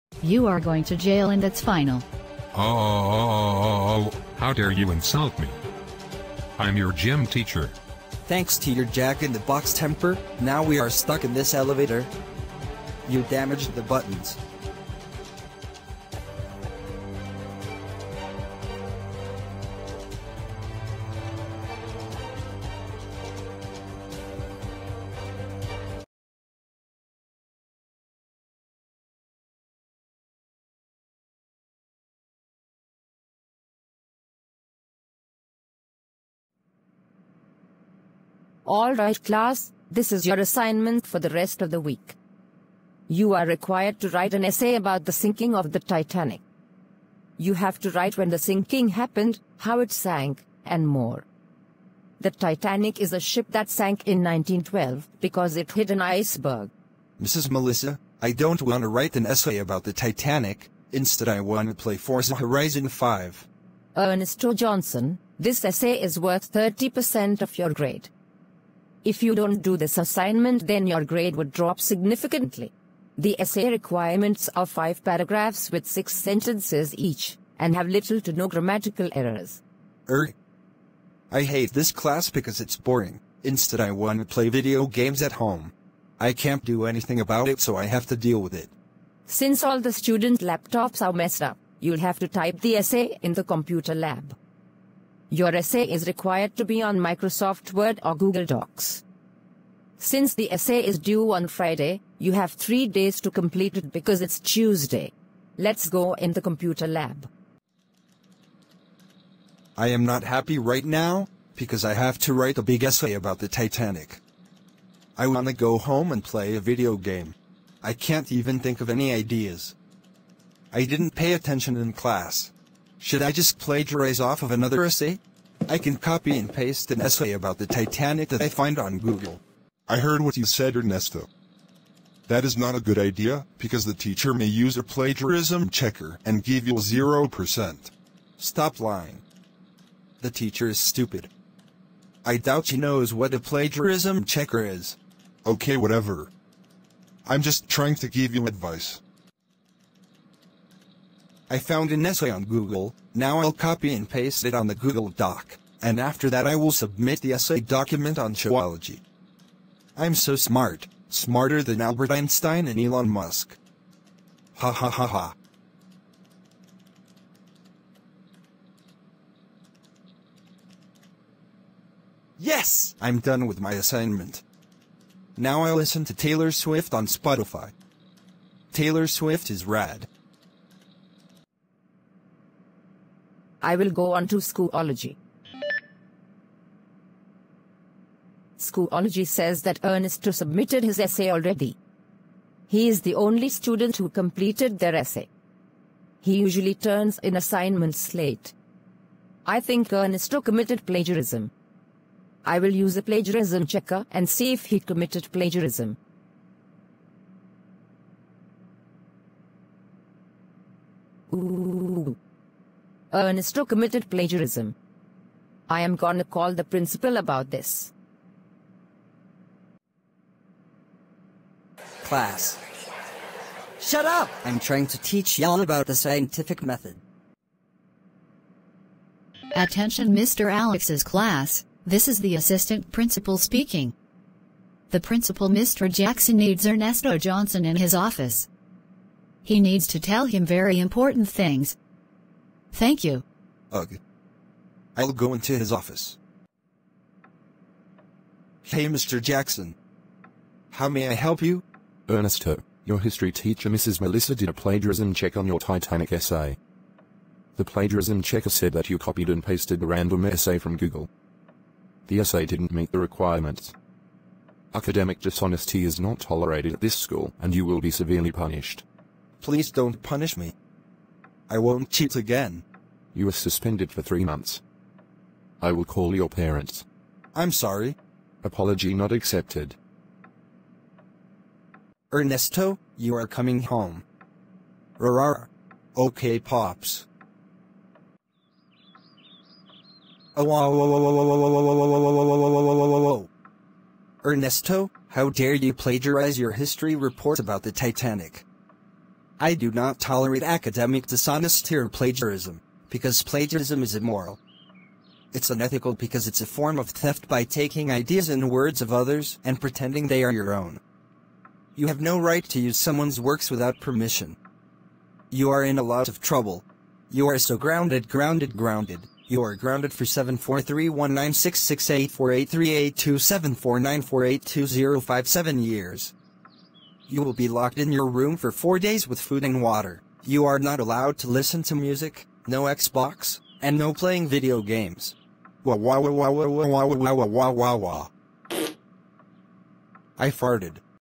Ernesto plagiarizes his history report from the internet/suspended/grounded
Ernesto copied and pasted an essay and said that it was his own.